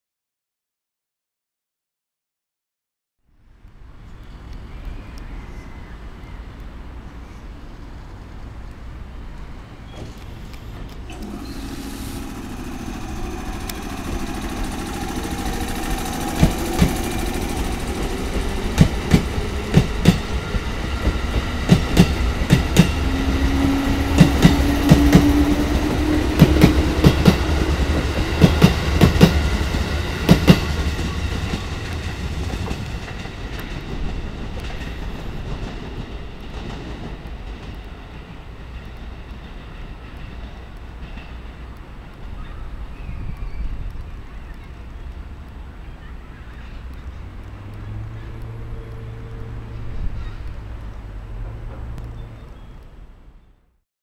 【尾道駅】213系C-04編成(官兵衛ラッピング)+213系C-06編成 【HD】2014.04.08
尾道駅にて
213系(6連)です。
オカC-04編成+オカC-06編成でした。
C-04編成には、NHK大河ドラマ　軍師官兵衛　のPRのラッピングが施されています。